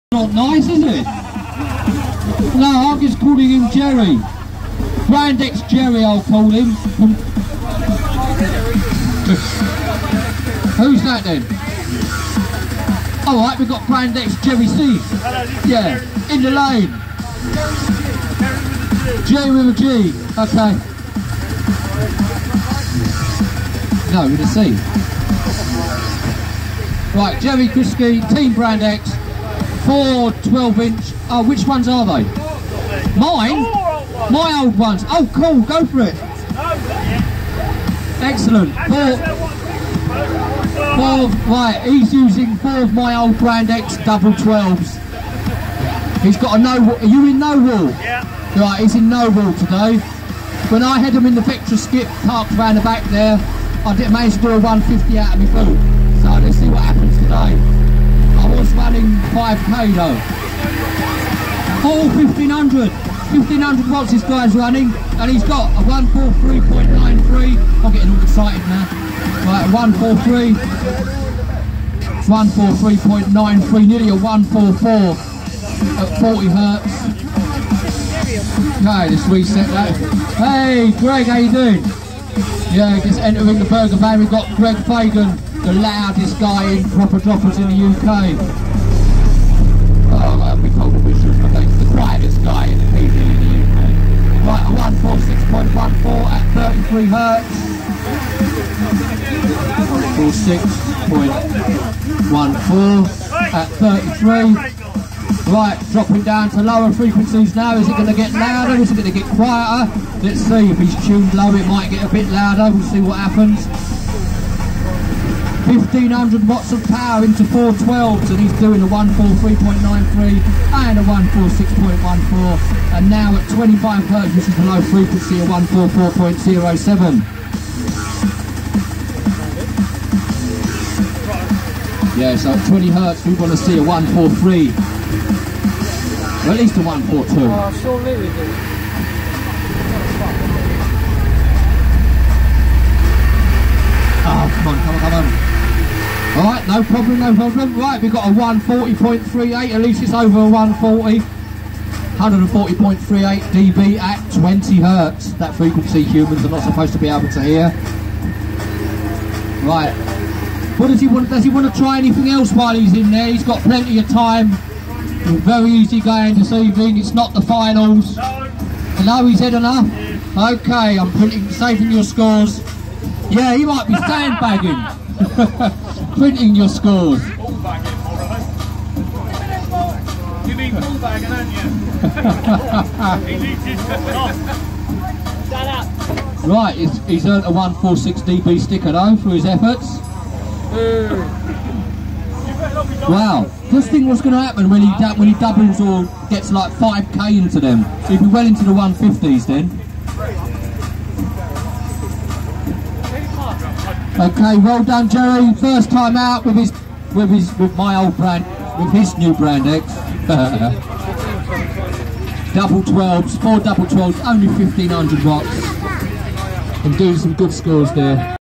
Team brand x audio my polo with 4 12s on 1500rms pd Christmas special king of the ring
Let me know what you think people thanks 👍🏻